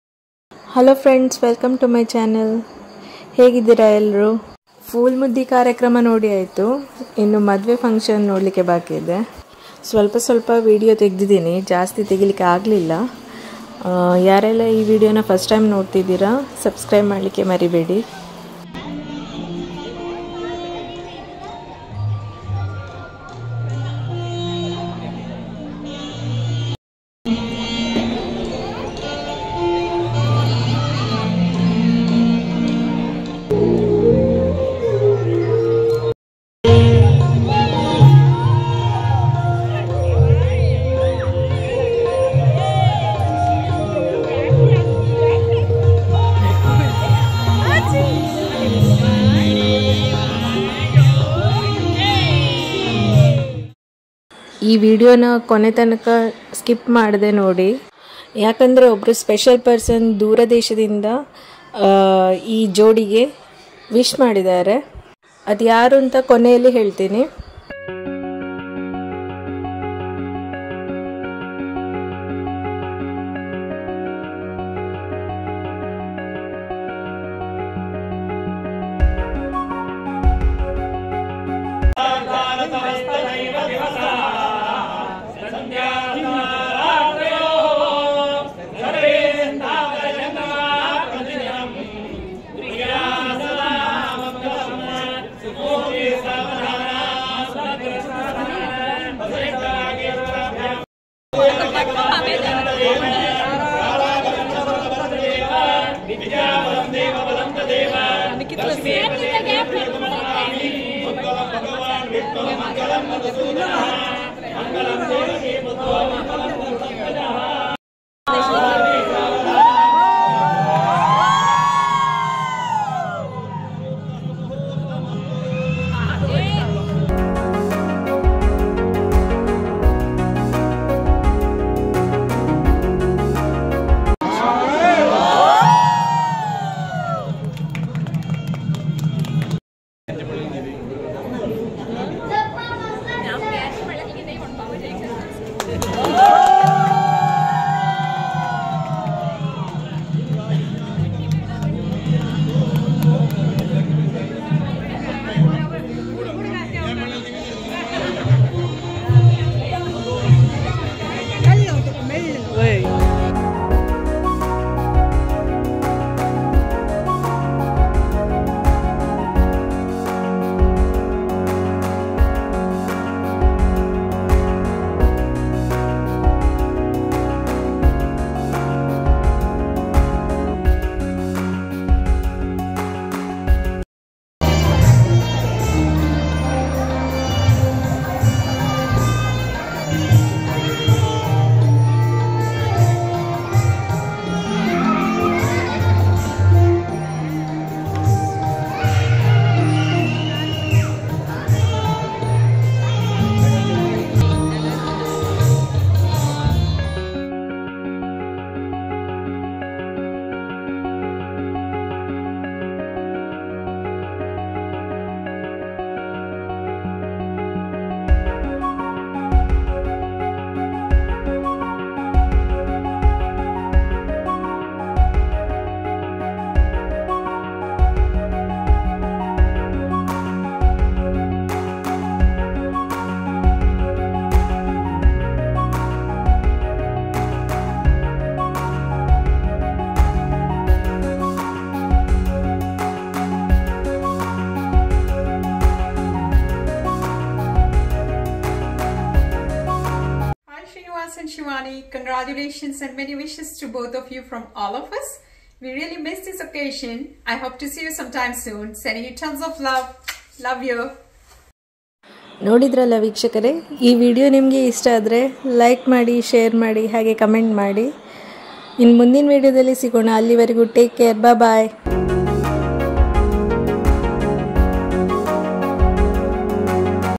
ಮದುವೆ ಸಮಾರಂಭ|ದೂರ ದೇಶದಿಂದ ನಮ್ಮ ಜೋಡಿಗೆ ಶುಭಾಶಯಗಳು ಹೇಳಿದ್ದಾರೆ ನೋಡಿkannadavlog
The traditional Indian wedding itself is a ritual of three separate events: the Sangeet and Mehendi, the ceremony, and the reception celebration.
Most wedding ceremonies involve an exchange of marriage vows by a couple.